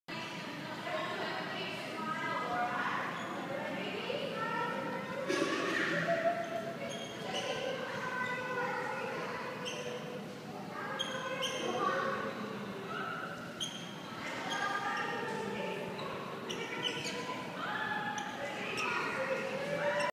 King penguin feeding its young
While at the Melbourne Aquarium, we got to witness a King penguin regurgitate his food to feed his young.